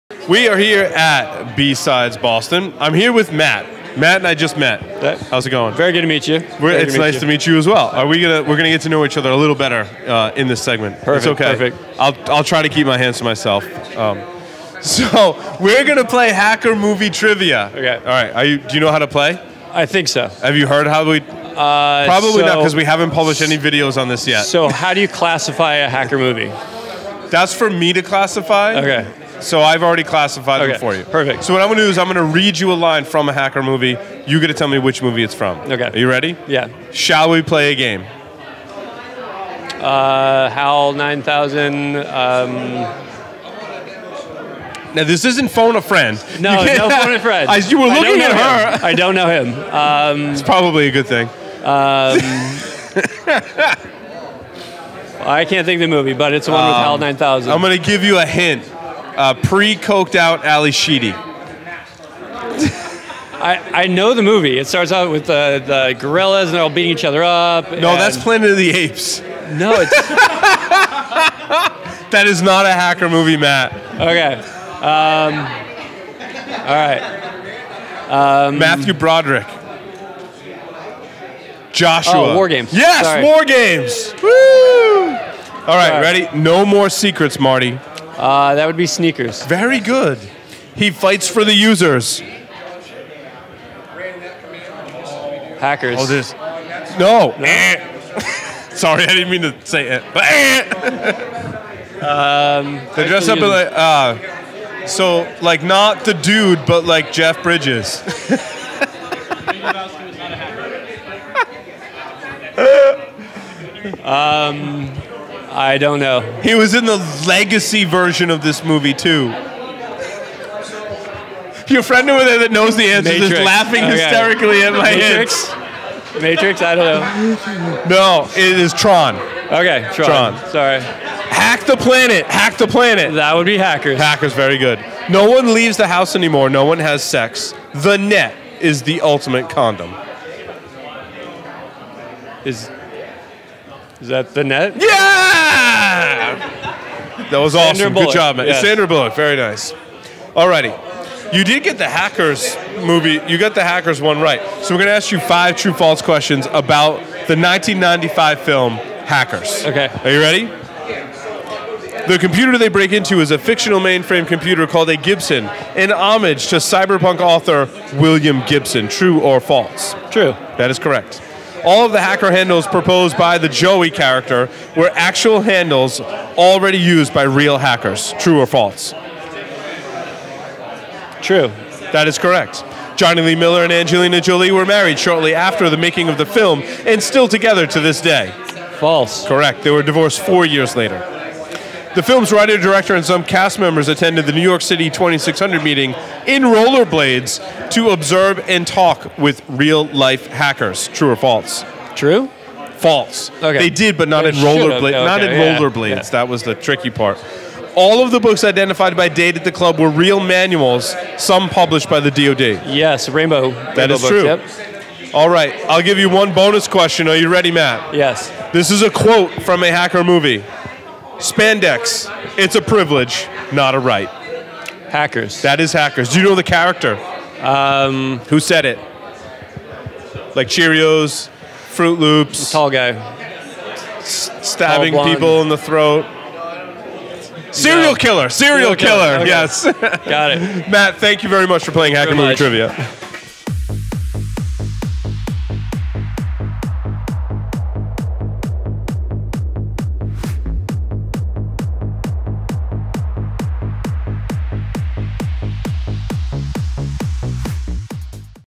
Matt Ploessel, Markley Group - Hacker Movie Trivia
Paul plays hacker movie trivia with Matt Ploessel at BSides Boston 2017! Matt is a Security Architect for the Markley Group, specializing in vulnerability research, cryptography, and more.